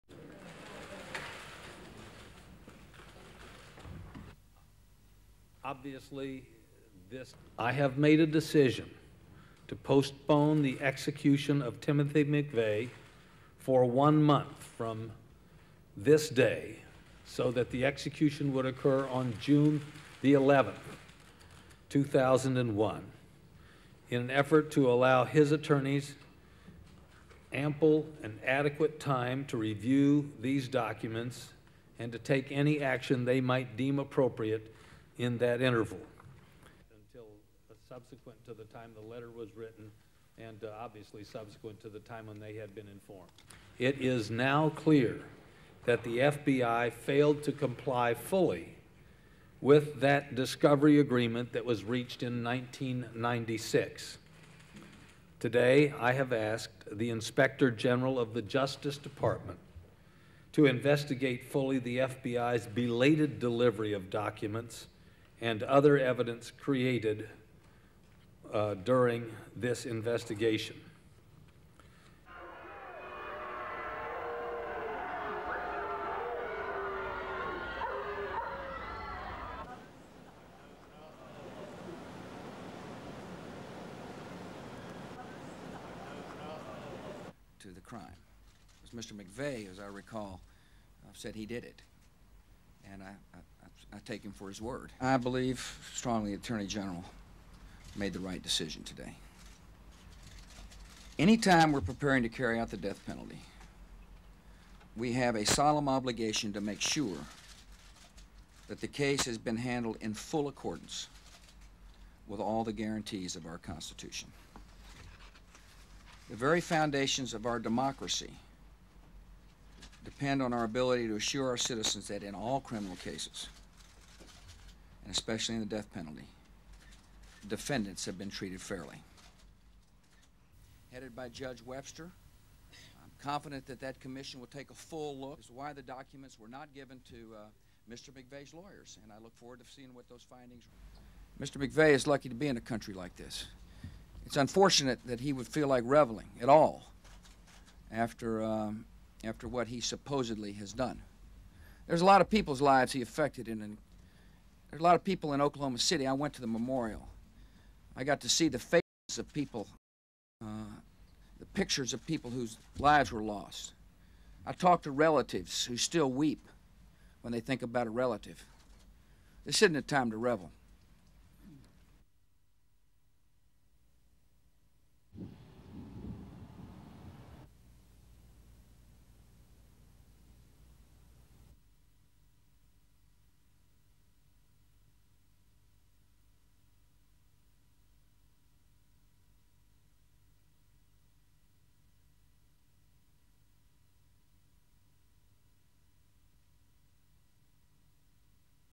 Wrap Ashcroft's announcement, Bush comments and file
Washington, D-C  - 11 May 2001 - POOL
1. Wide-shot of John Ashcroft walking into room
2. Cutaway of reporters
3. SOUNDBITE: (English) John Ashcroft, U-S Attorney General
"I have made a decision to postpone the execution of Timothy McVeigh for one month from this day, so that the execution would occur on June 11, 2001, in an effort to allow his attorneys ample and adequate time to review these documents and to take any action they might deem appropriate in that interval."
4. Cutaway of reporters
5. SOUNDBITE: (English) John Ashcroft, U-S Attorney General
"It is now clear that the F-B-I failed to comply fully with the discovery agreement that was reached in 1996. Today, I have asked the inspector general of the Justice Department to investigate fully the F-B-I's belated delivery of documents and other evidence created during this investigation."

Oklahoma City - APTN File
6. McVeigh after arrest
7. Aftermath of the Oklahoma City bombing

Washington, DC - 11 May 2001 - POOL
8. Wide-shot of President George W. Bush talking in the briefing room
9. Cutaway of press
10. SOUNDBITE: (English) George W. Bush, U-S President
"I believe strongly that the Attorney General made the right decision today. Anytime that we are preparing to carry out the death penalty, we have a solemn obligation to make sure that the case has been handled in full accordance with all the guarantees of our Constitution. The very foundations of our democracy depend on our ability to assure citizens that in all criminal cases, especially in the death penalty, the defendants have been treated fairly."
11. Cutaways of press
12. SOUNDBITE: (English) George W. Bush, U-S President
"Mr. McVeigh is fortunate to be in a country like this.  It's unfortunate that he would feel like reveling at all, after what he supposedly has done. There's a lot of peoples' lives he's affected. There are a lot of people in Oklahoma City. I went to the memorial. I got to see the faces of people, the pictures of people who's lives were lost. I talked to relatives who still weep when they think about a relative. This isn't a time to revel."

Terra Haute, Indiana - POOL
13. Exteriors of the federal prison
14. Shots of execution chamber

STORYLINE:

U-S Attorney General John Ashcroft on Friday postponed next week's execution of convicted Oklahoma City bomber Timothy McVeigh until June 11th.

The postponement comes a day after the F-B-I revealed it had failed to turn over thousands of documents to McVeigh's defence team.

The execution had been scheduled for Wednesday May 16 in Terre Haute, Indiana.

Ashcroft acknowledged that McVeigh, by his own admission, is guilty of the Oklahoma City bombing but after learning of the existence of the withheld documents, Ashcroft said it was his responsibility to protect the integrity of the justice system.

Ashcroft also ordered an investigation into the F-B-I's failure to turn over thousands of documents to McVeigh's defence team.

The Justice Department on Friday handed McVeigh's lawyers more than three-thousand documents it said should have been provided during the discovery phase of his 1997 trial in Denver.

Legal analysts said the mistake could delay what would be the first federal execution since 1963, though it was unlikely to overturn McVeigh's conviction.

U-S President George W. Bush backed Ashcroft's decision to postpone McVeigh's execution, saying it was important for the nation to make sure death penalty cases are handled fairly.